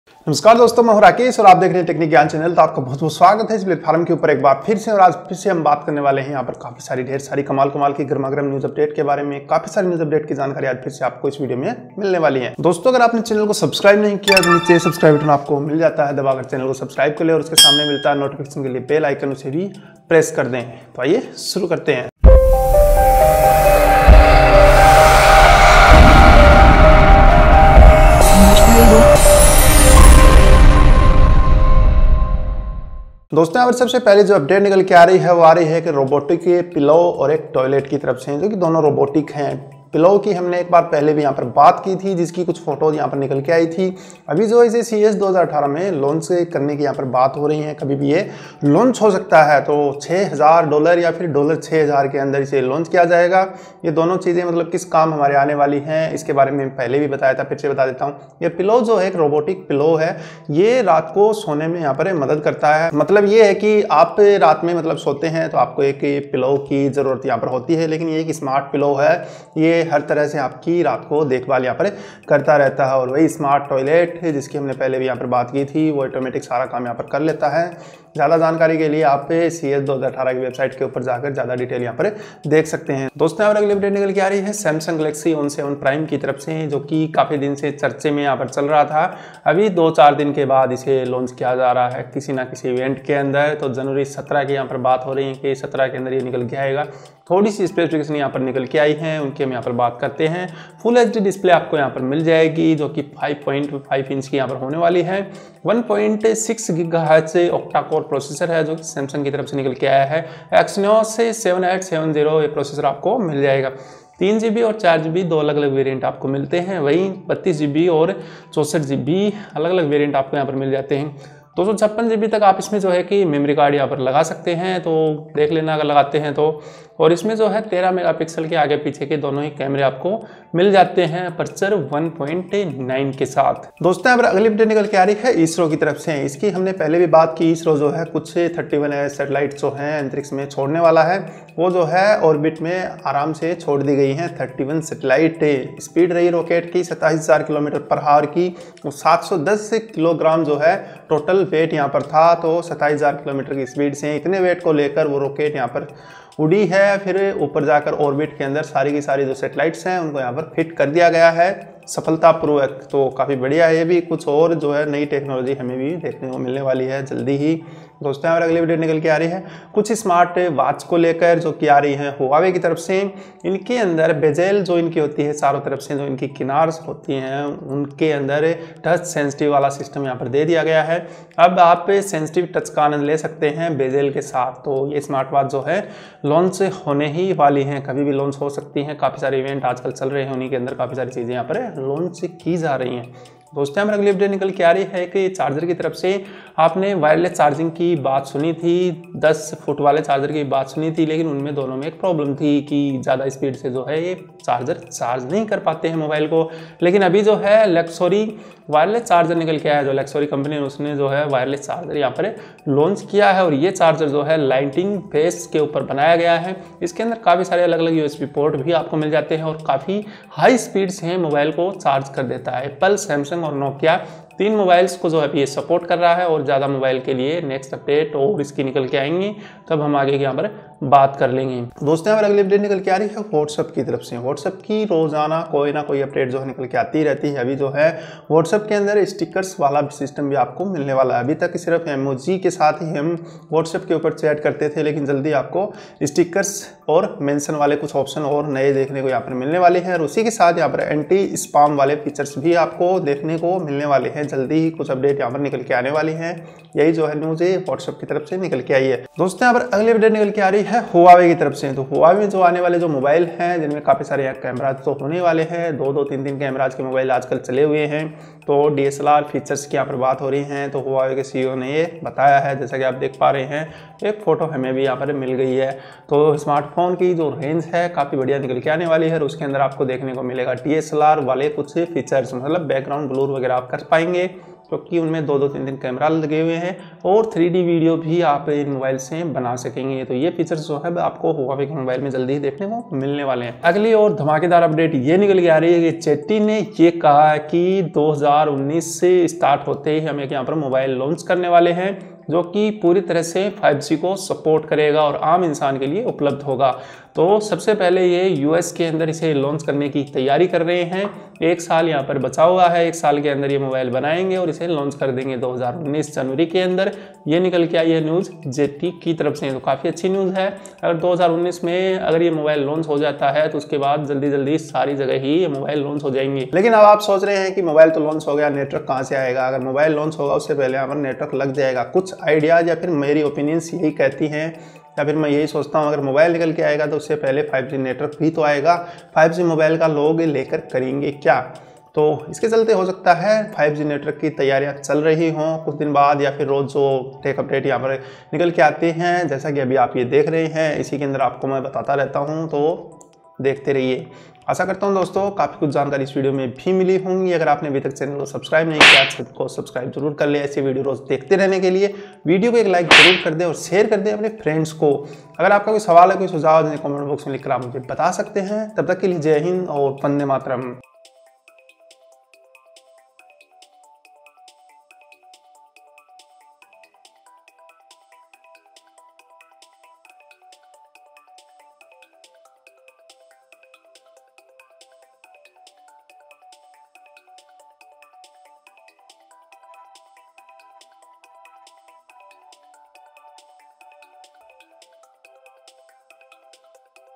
Tech Up's #121- ZTE 5G Phone, $6000 Pillow, Galaxy On7 Prime, ISRO, Qi Charger, whatsapp, DSLR
HINDI - Tech Up's #121- ZTE 5G Phone, $6000 Pillow, Galaxy On7 Prime, ISRO, Qi Charger, whatsapp, DSLR

1. From a $6,000 toilet to a robotic pillow, here are the weirdest gadgets showcased at the event
2. Samsung Galaxy On7 Prime likely to launch on January 17
5.5-inch full-HD (1080×1920 pixels)
1.6GHz octa-core Exynos 7870 processor
3GB of RAM and 32GB internal storage,
 another with 4GB of RAM and 64GB built-in storage.
 expandable up to 256GB via microSD cards.
13-megapixel camera of aperture f/1.9 on the front and back.
3. ISRO drops 31 satellites in the orbit at a speed of 27,000km/h
weighs 710 kilograms
4. Smartwatches With Touch-sensitive Bezels Coming From Huawei
5. LXORY Wireless Charger Is the First Lightning Based Qi Charger
6. WhatsApp for Android To Soon Get Stickers, Mentions and Anti-Spam Features
7. Huawei Wants to Bring DSLR Features to Its Smartphone Range
8. ZTE Plans to Launch First 5G Smartphone in US in Early 2019